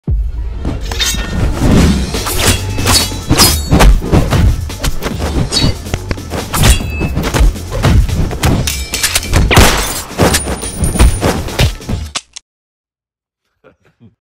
The Tactical Combat Simulator: Splack
The Tactical Combat Simulator Series: Featuring Jean Robert. In this episode of the mini series, Tony Surphman fights Jean Robert. This combat training includes hand to hand combat choreography as well as a Knife fighting tactical stunt choreography.